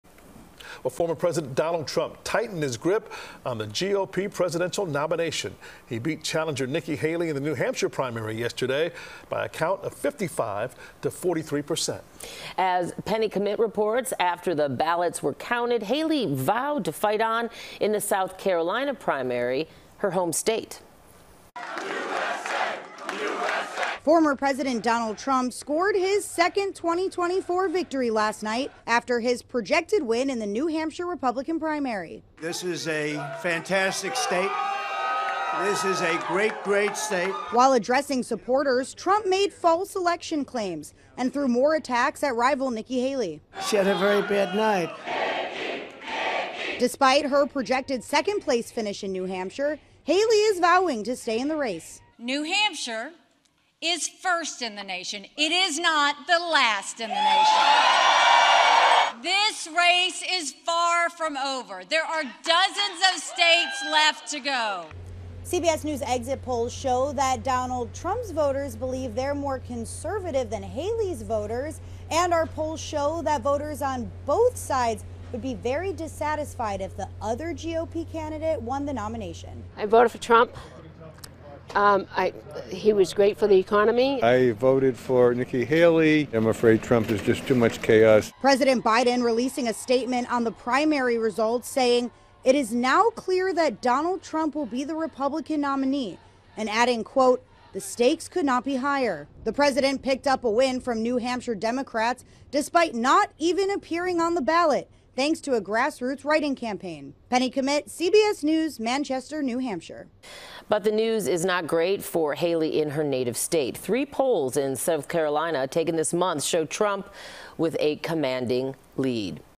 Former President Trump wins New Hampshire primary
Former President Donald Trump tightened his grip on the GOP presidential nomination on Tuesday, beating former U.N. Ambassador Nikki Haley in the New Hampshire primary election by a 55 to 43 percent margin. After the ballots were counted, Haley vowed to fight on in the South Carolina primary in her home state.